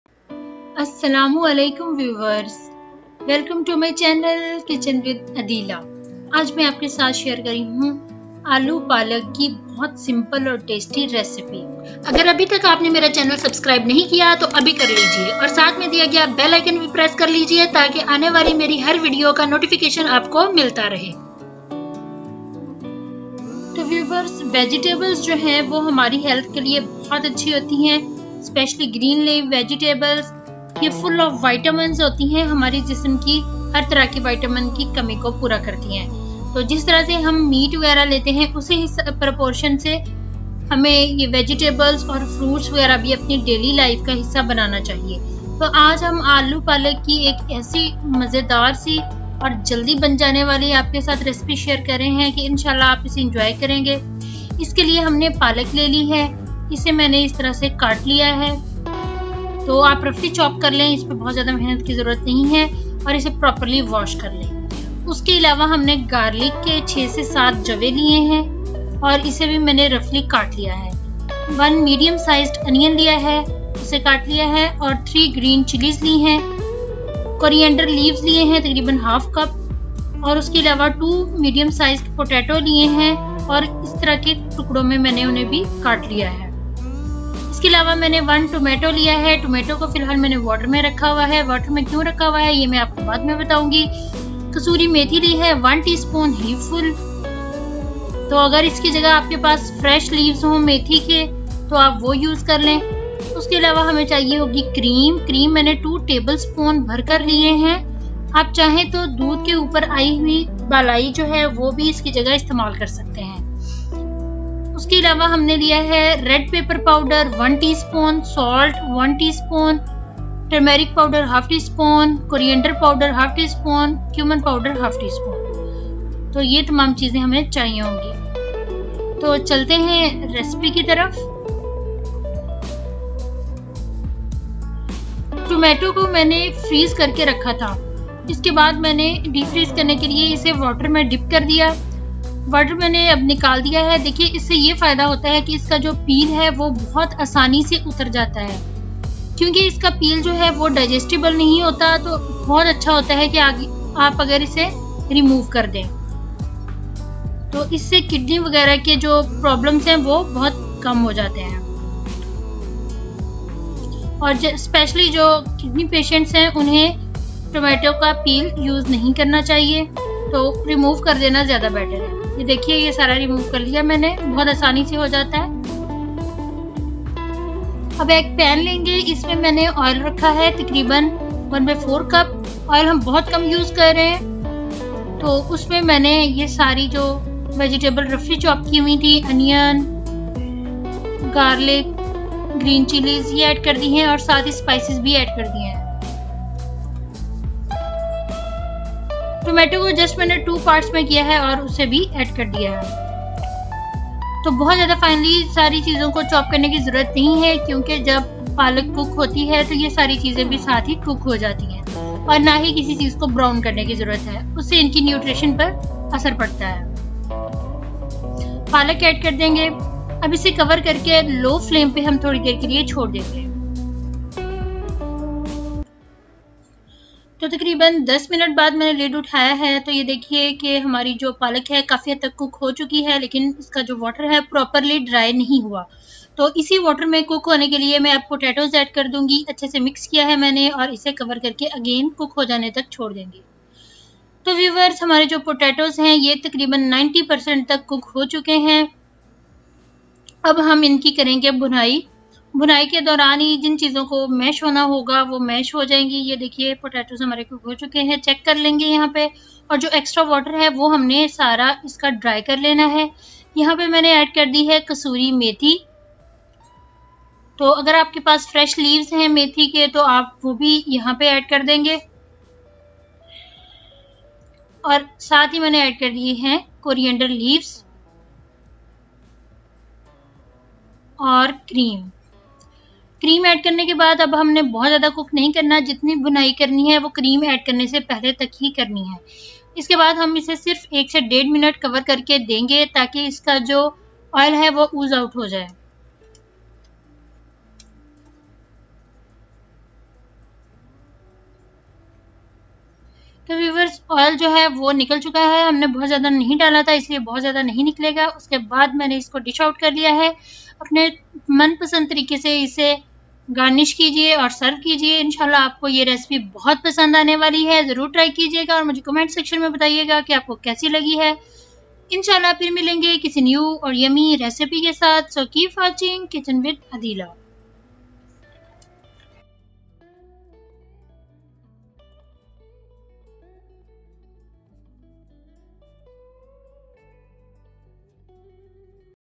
Aloo Palak Recipe | मिनटों में बनाएं स्वादिष्ट आलू पालक की सब्ज़ी | Easy Aloo Palak Ki Sabzi
Potato spinach curry very tsaty
Must try
Yummy
Mouth watering

my channel for get new original recipe